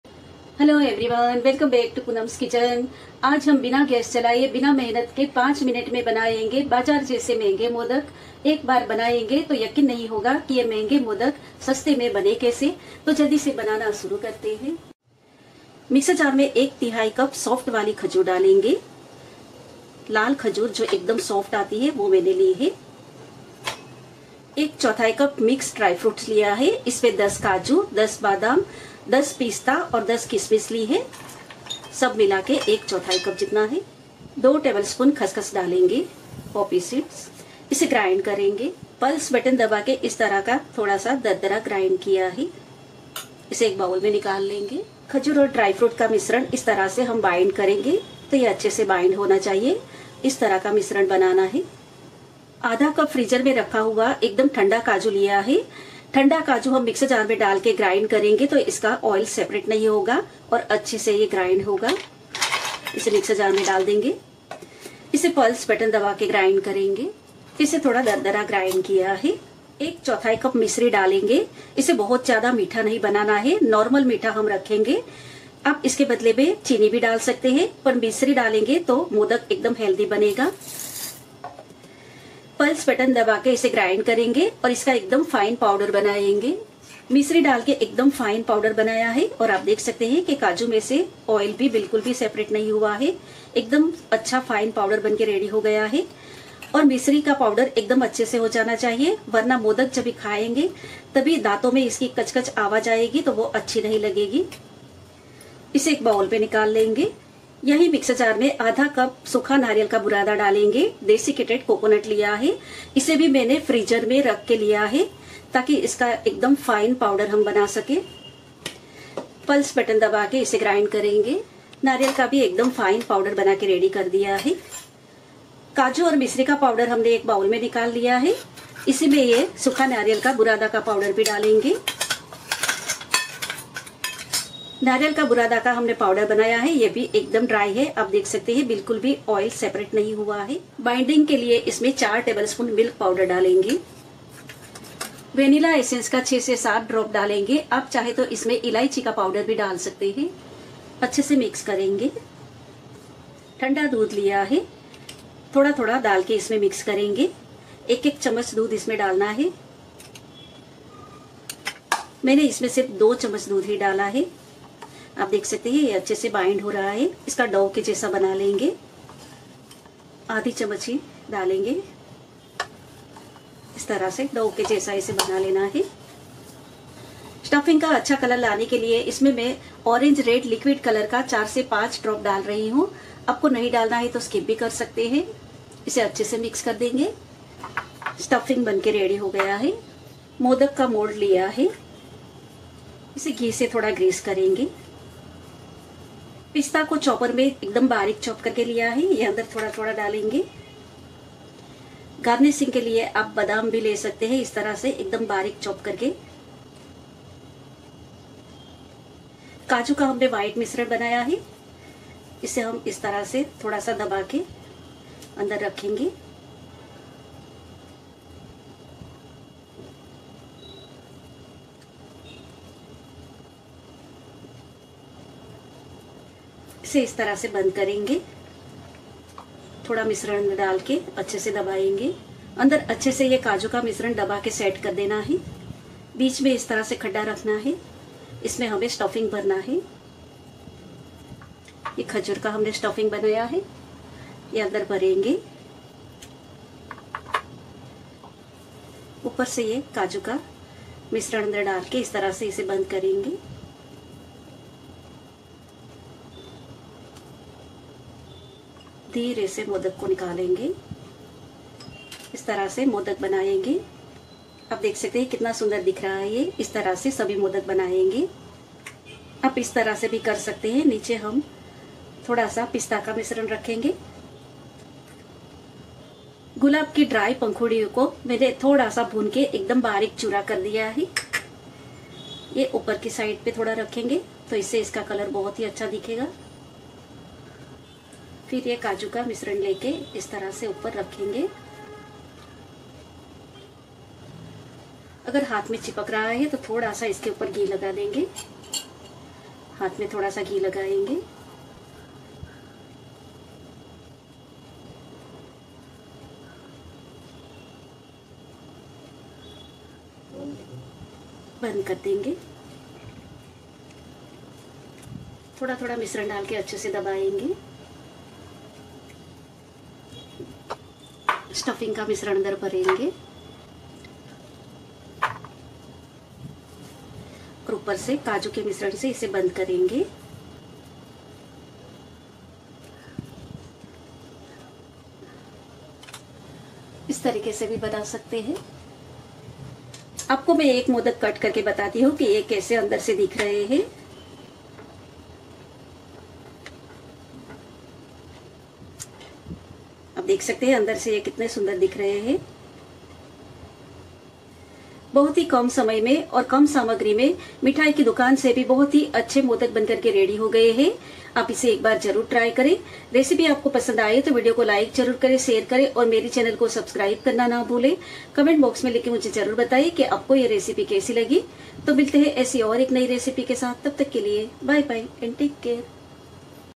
5 Minute Quick Simple Modak Recipe without Cooking | Ganesh Chaturthi Special Modak| 5 मिनट में मोदक
Poonam’s Kitchen presents Rich Modak Recipe using basic ingredients in only 5 minutes. This is a quick and simple modak for Ganeshji that can be prepared without cooking.
This step by step video for modak will enable you to prepare these Modak in an easy manner.
Do try this Modak recipe at home!

Modak Recipe Ingredients

1/3 cup Soft Khajur (Dates)
1/4 cup Mix Dry Fruit(10Kaju,10Pista,10Badam,10Kismis)(Cashew nut,Pistachio,Almond,Raisins)
2 Tbl sp Khas Khas (Poppy seed)
1/2 cup Cold Kaju(Cold Cashew nut )
1/4 cup Misri(Candy Sugar)
1/2 cup Sukha Nariyel Ka Bhurada (Desicated coconut)
4 Tbl sp Milk Powder
4 to 5 Drops Vanilla Essence
2 Tsp Cold Milk
4 to 5 Drops Orange Liquid food colour
Pista finely chopped (Pistachios)
Roasted Gulab ki Pankhudi(Rose Petal)

Enjoy Amazing Modak Recipe with your Family and friends